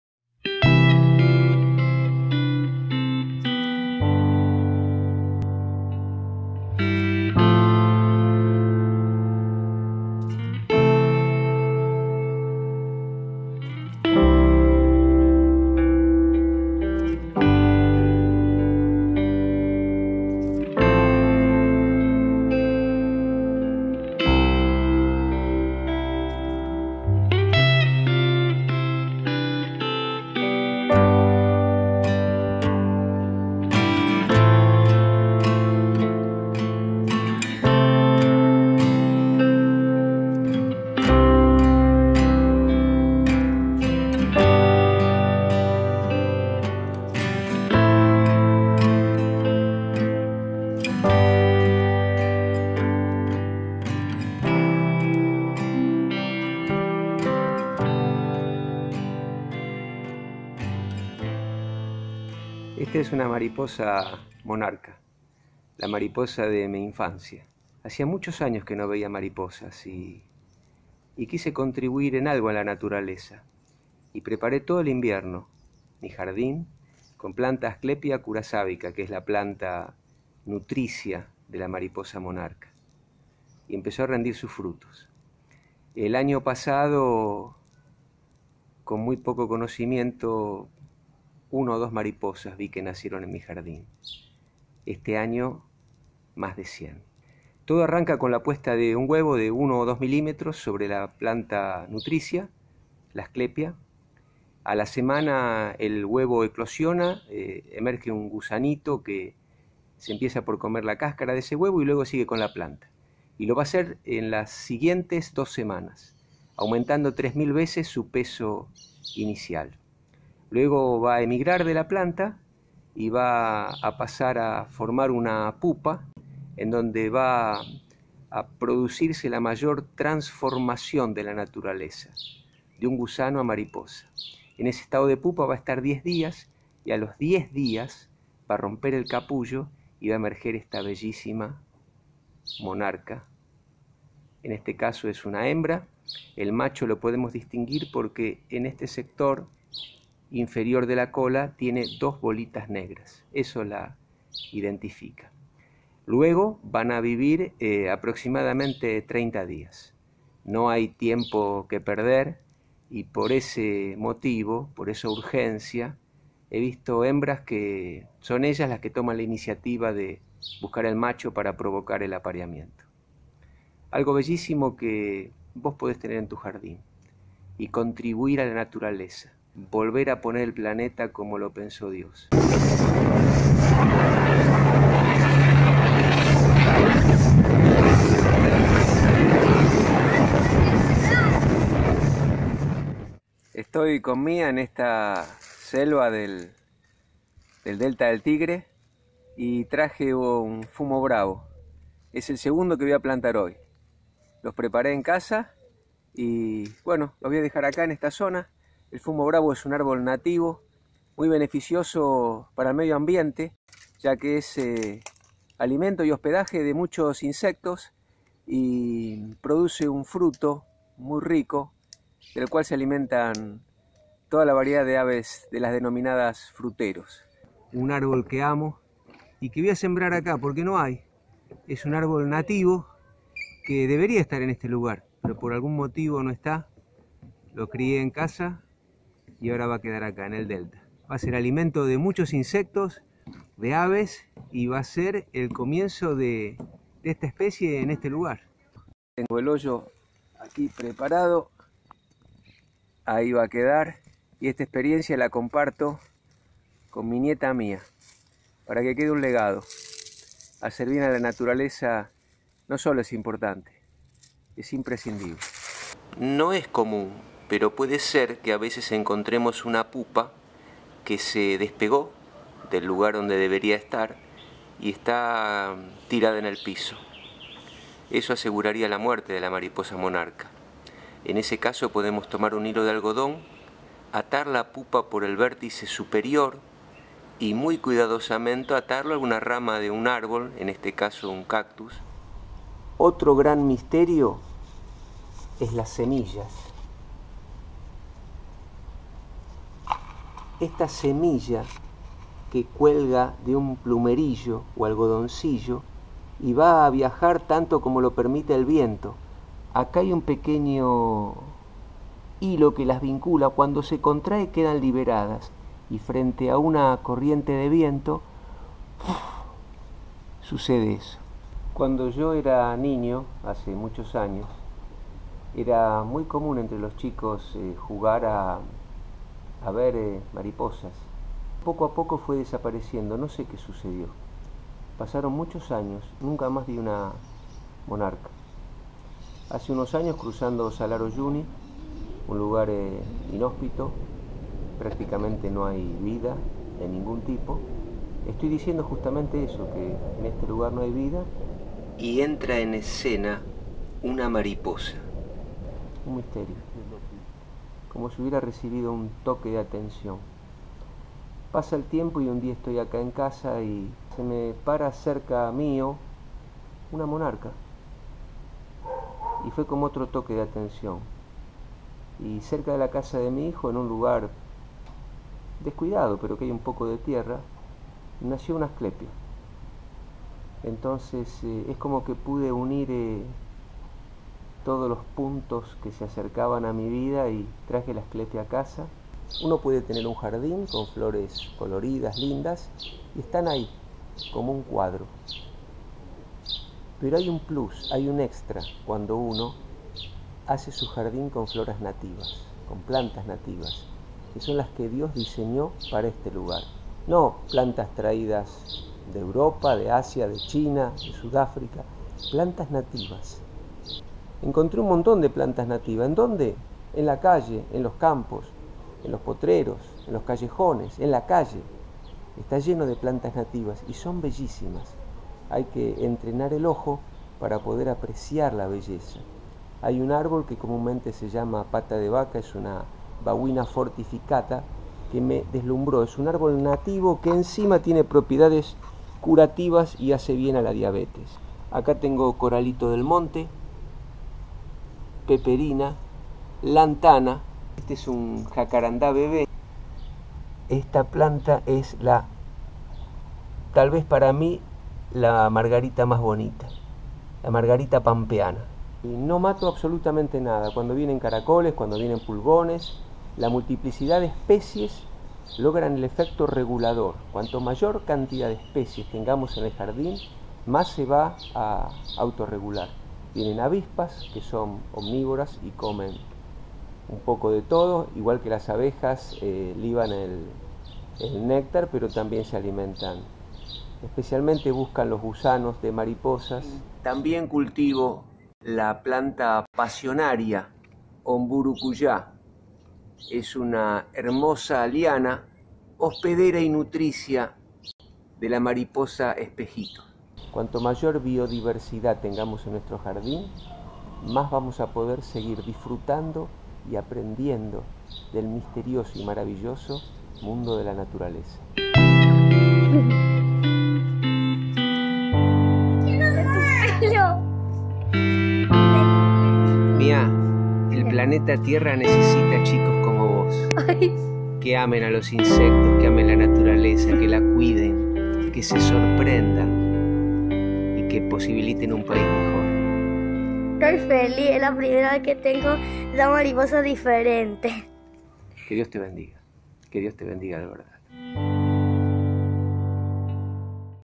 Mariposa Monarca - DANAUS ERIPPUS - Monarch butterfly
El asombroso ciclo de vida de la mariposa Monarca. Plantas hospederas y nutricias. Semillas diseñadas para cruzar las fronteras de lo imposible. Colores que advierten veneno en sus fluidos. Toda la naturaleza conectada y sostenida a partir del milagro de la vida.